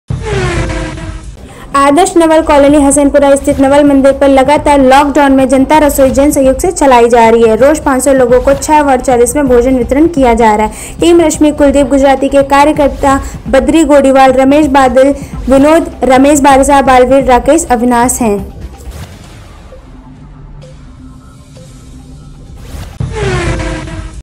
Jaipur | टीम रश्मि कुलदीप गुजराती द्वारा जरूरतमंद लोगों के लिए जनता रसोई | SNI NEWS
आदर्श नवल कालोनी हसनपुरा  स्थिति नवल मंदिर पर  लगातार लॉकडाउन   में जनता रसोई  जन सहयोग से चलाई जा रही है रोज 500 लोगों को क्षयवार्ड 40 में  भोजन वितरण किया जा रहा है टीम रश्मि कुलदीप गुजराती के कार्यकर्ता बद्री गोडिवाल  , रमेश बादल, विनोद, रमेश बारासा, बलवीर , राकेश, अविनाश है